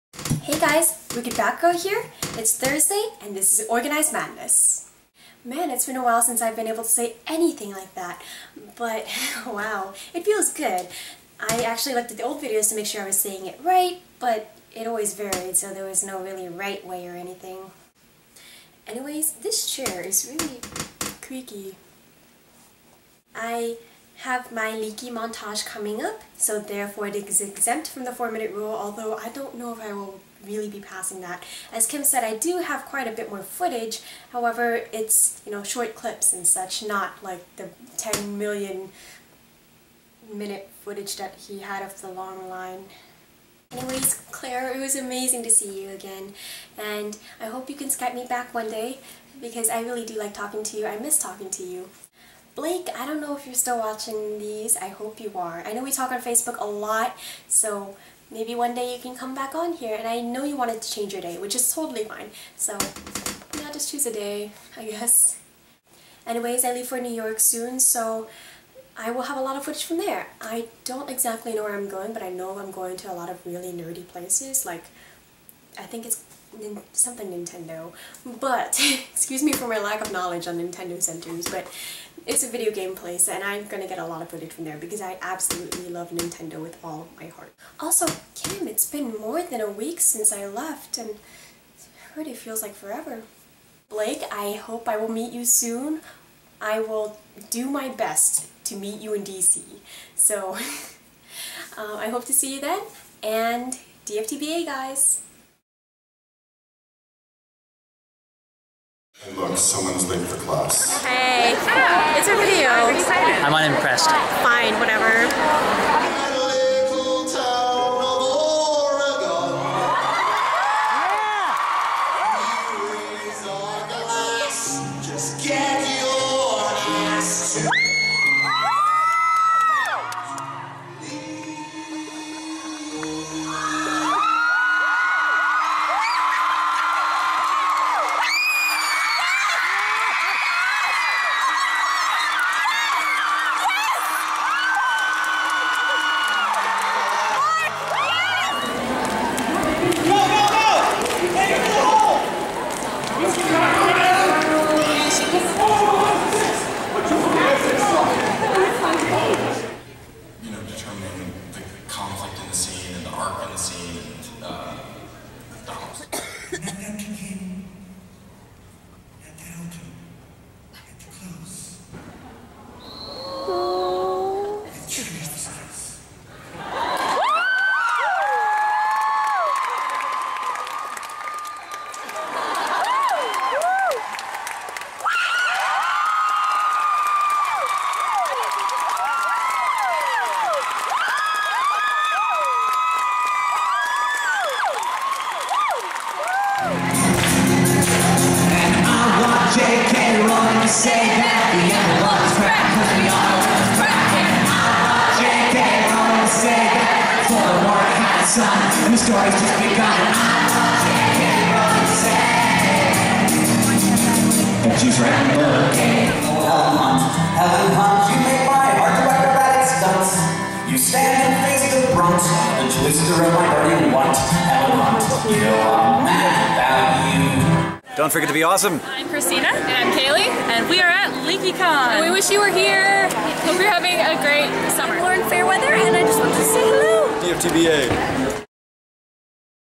OrganizedMadness ~ LeakyCon 2013 with Kimm!
Hey guys!
This is my first video in a while and I hope you all are doing well.  I hope we can get OM started up again.  Let's do this thing! :D
I hope you enjoy this.  The official montage will go up on my channel when I come back from NYC.  See you all then!
Also, Kimm, thanks for making this video public.
I hope. :P (You're Welcome ^.^ -Kimm)

~Dom